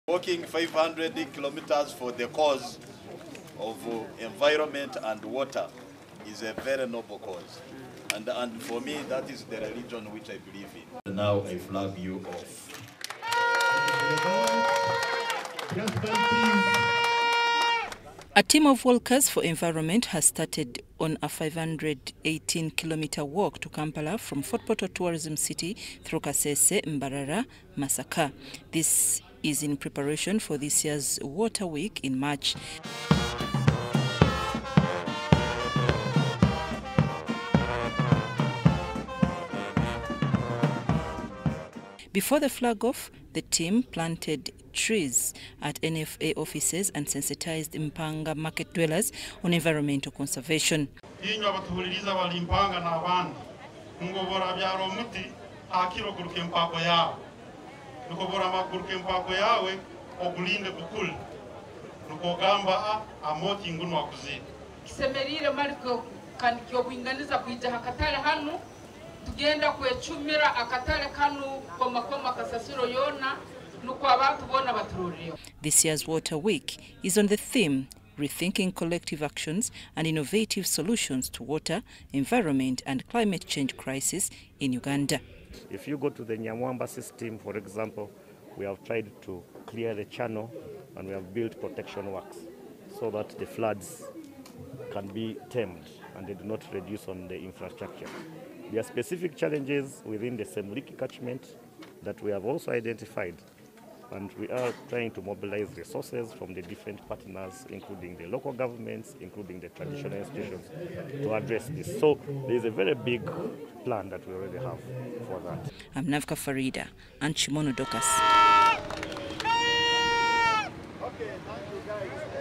MINISTRY OF WATER AND ENVIRONMENT LAUNCHES 500KM ENVIRONMENTAL SENSITIZATION TREK
Ministry of water and environment has dispatched a team of walkers for environment to a sensitization campaign that encompasses trekking over 500KM from Fort Portal to Kampala via Kasese, Mbarara and Masaka
Uganda will in March this year observe another water week with the aim of sensitizing communities on the importance of environmental conservation and optimal use of natural resources.